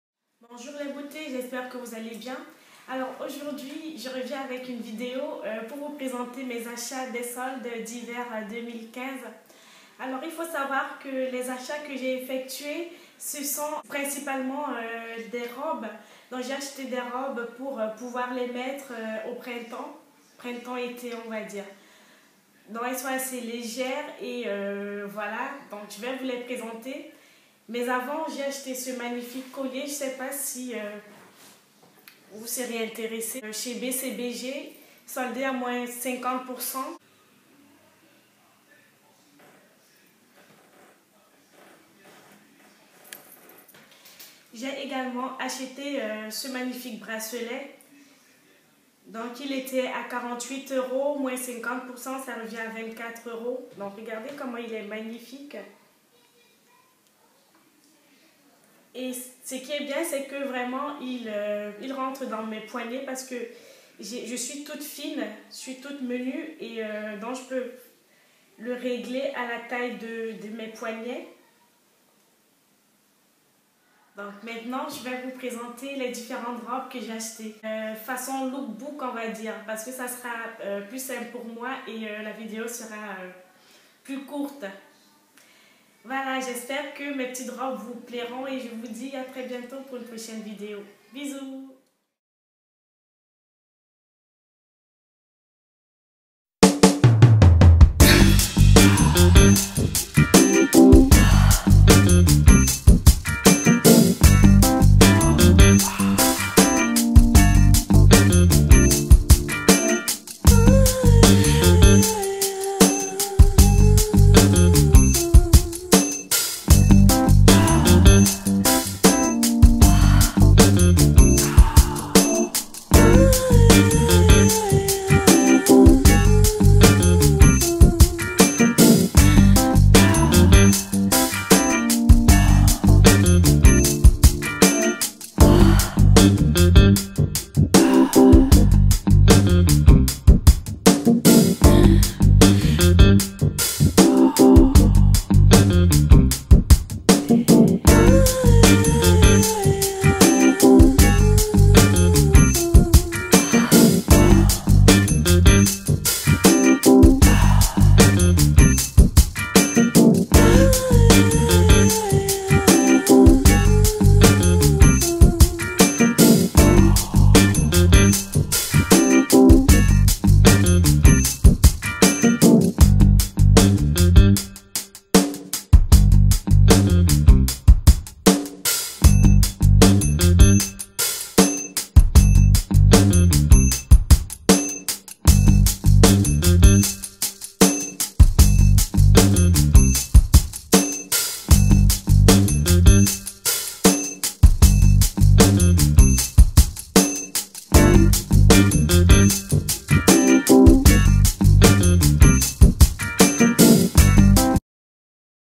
Mes soldes d'hiver façon lookbook
Bonjour les beautés voici mes petits achats effectués pendant les soldes de cet hiver , j'espère qu'ils vous plairont.

Gros bisous à tous